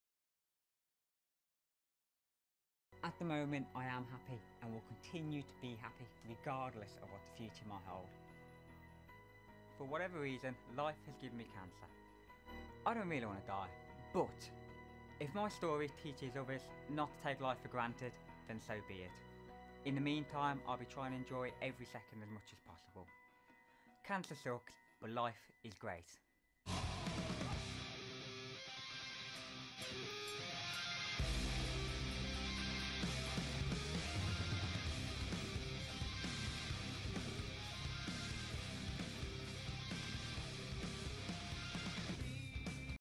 Teenage cancer sufferer Stephen Sutton dies
Cancer sufferer Stephen Sutton, who raised millions for charity, has died. Watch one of the videos he made before his death here. .Sign up for Snowmail, your daily preview of what is on Channel 4 News, sent straight to your inbox,